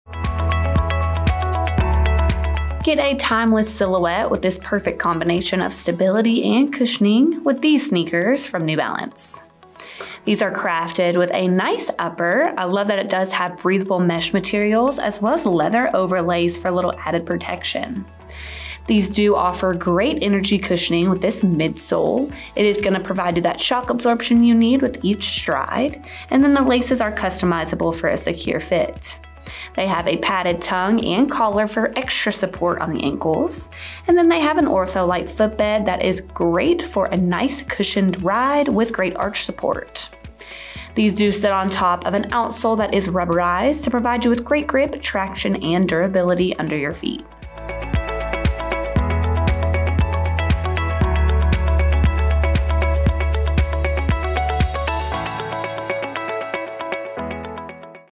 New Balance Made in US 990v5 SKU: 9188432
The perfect combination of stability and cushioning fuse together in the newest form of the New Balance® 990v5 running-inspired lifestyle shoes. The timeless silhouette and unbeatable reputation will take your farther than ever before.
Predecessor: M990V4.
Support Type: Neutral.
Cushioning: High energizing cushioning.
Surface: Road.
Differential: 12 mm.
Updated upper features breathable mesh and pigskin-leather materials.
Lace-up closure.
Plush tongue and dual-density foam collar.
Breathable fabric lining offers a great in-shoe feel.
Removable foam insole.
ENCAP® PU ring with EVA core wraps the lateral mid-foot through the heel, and around the medial mid-foot for enhanced stability.
Ortholite® premium footbed provides cushioning and arch support.
Blown rubber outsole.
Made in the U.S.A. from imported materials.